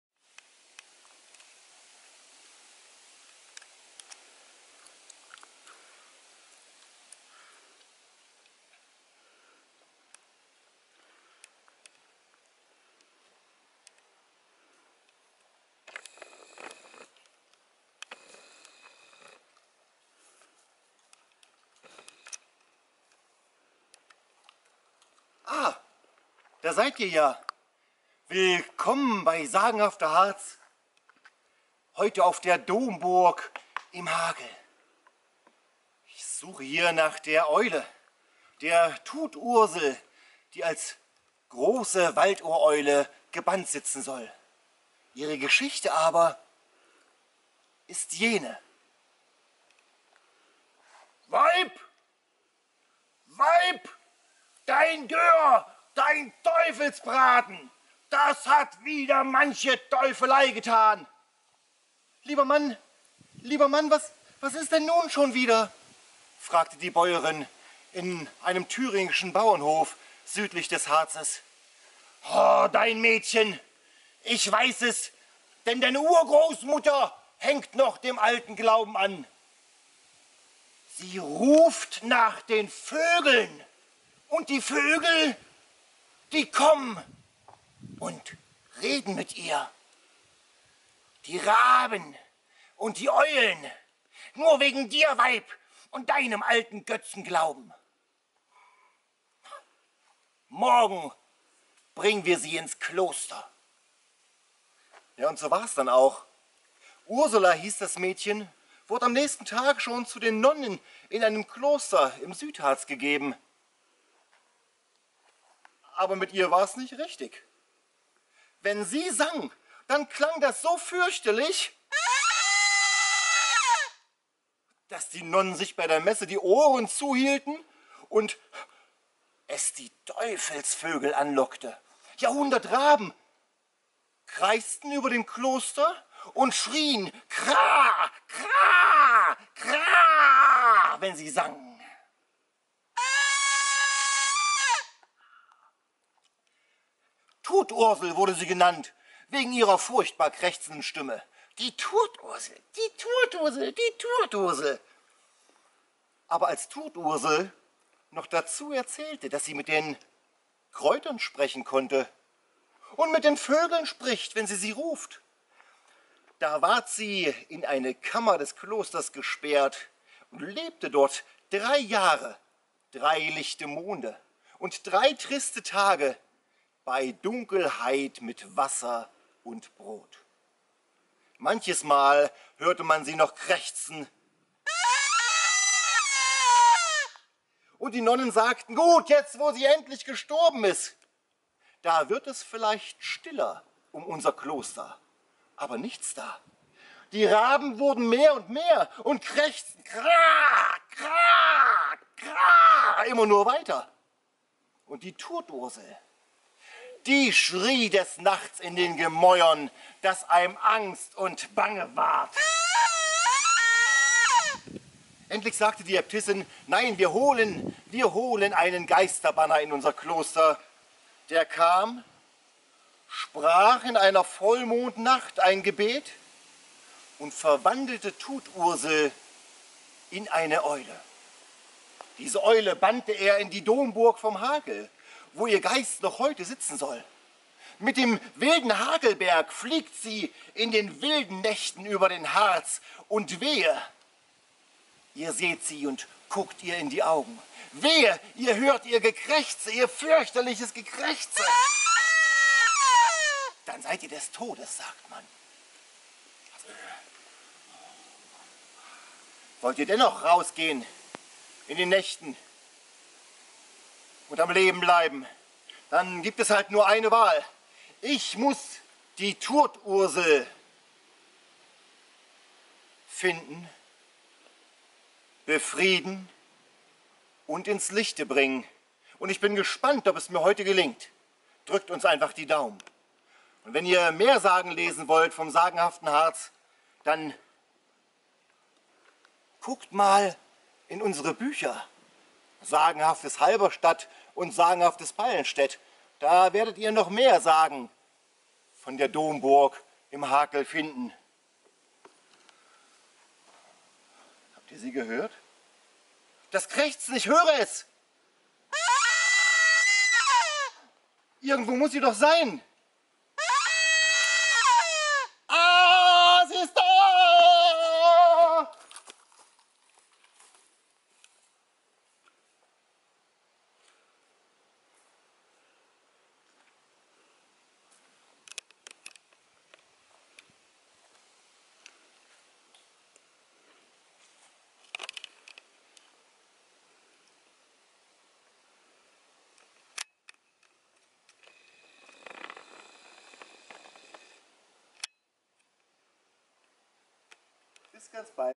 Sagen im Harz: Die Tut-Ursel auf der Domburg im Hakel
In der Domburg ist eine Eule gebannt, der besser nicht zunahe kommen sollte. Was es mit diesem Nachtvogel auf sich hat, erfahrt ihr in dieser Sage!